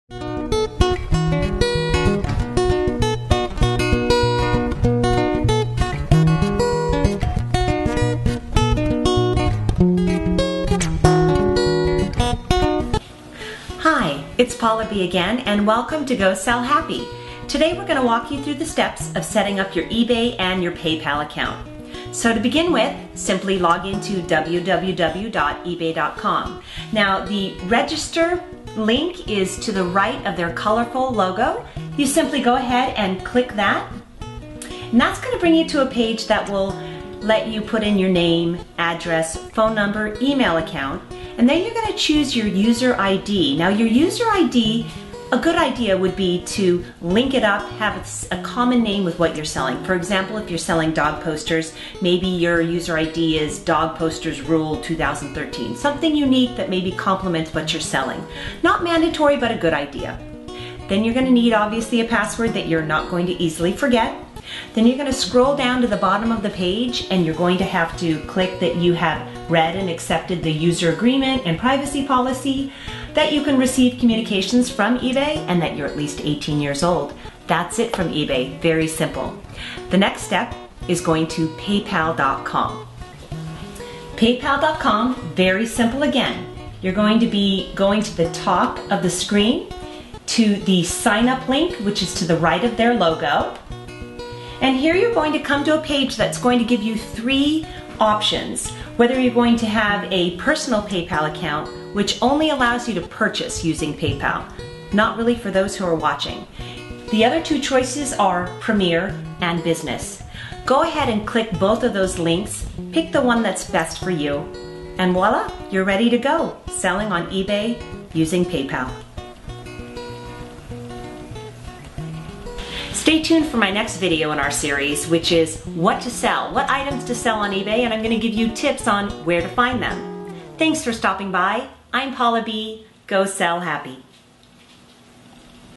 eBay Basics - Setting up eBay and Paypal Accounts with Paula B
Setting up eBay and Paypal account and start selling on eBay.